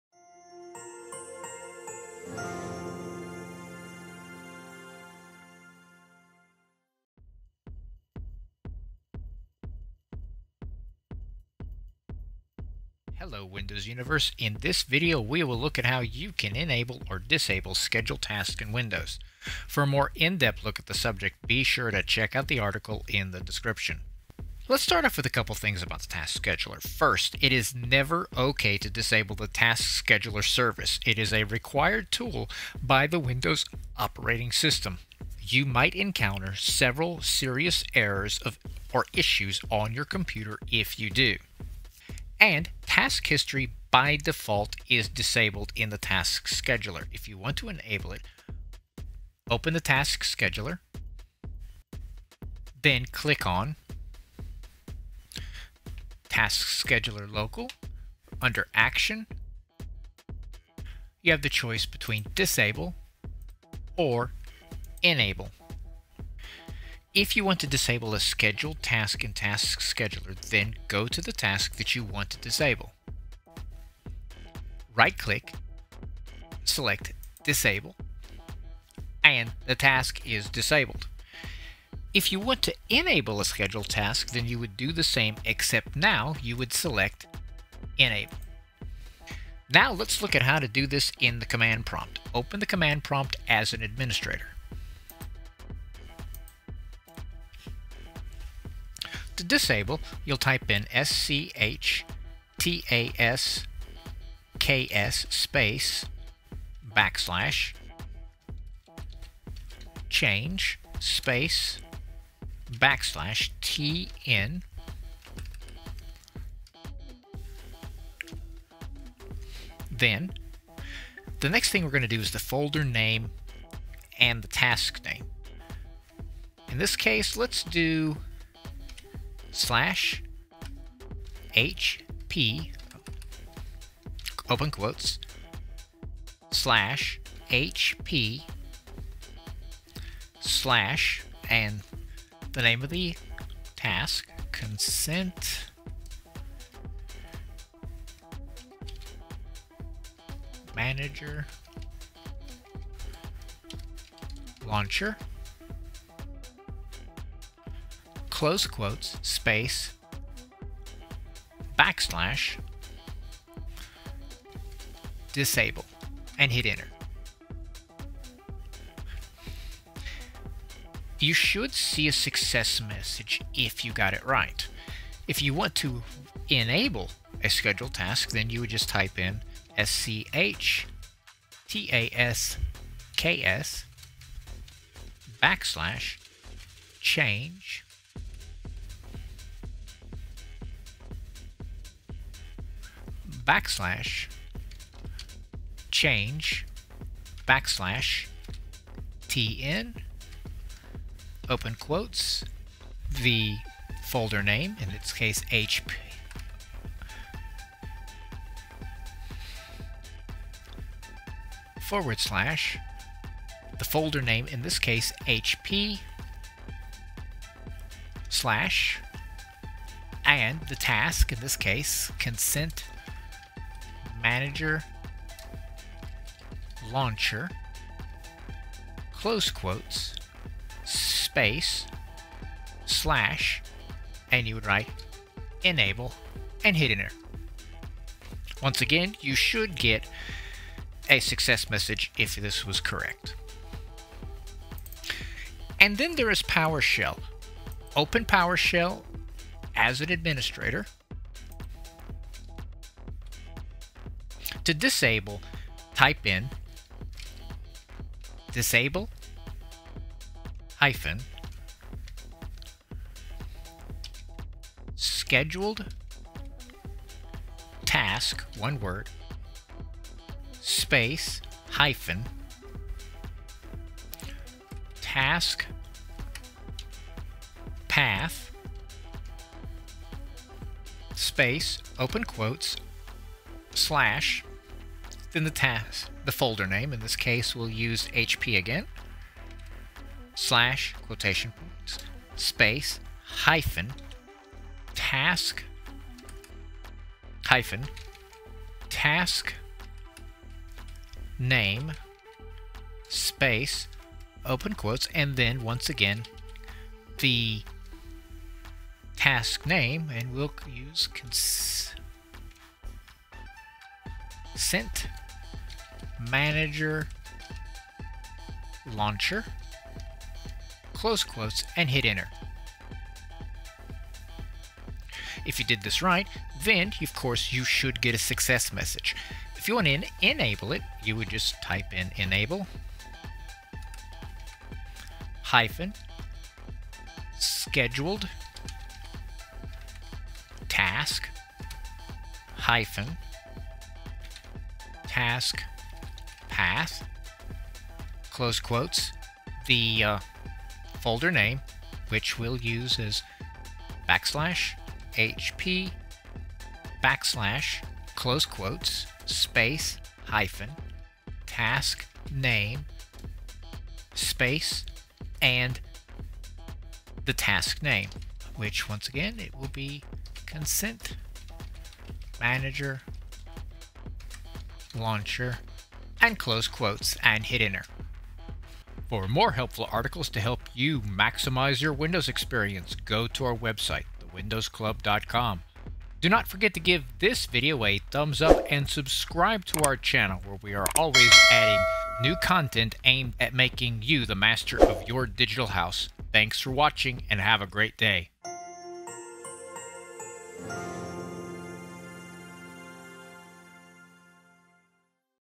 How to enable or disable Scheduled Task in Windows 11/10
Task Scheduler is a built-in component in Windows OS that lets the user schedule tasks (like Scheduling a Shutdown or Restart) for a particular date and time. On its interface, you can see the status of every scheduled task.

If you see the status as Ready, it means that the particular task is enabled. On the other hand, if it shows the status as Disabled, that means the particular task has been disabled. Users can also change the status of the scheduled tasks manually.

You can enable or disable the scheduled tasks in three ways. These are:

1] Using Task Scheduler

2] Using Command Prompt

3] Using Windows PowerShell

Timecodes:
0:00 INTRO
0:24 A FEW NOTES
1:06 USING TASK SCHEDULER
1:29 USING THE COMMAND PROMPT
4:04 USING POWERSHELL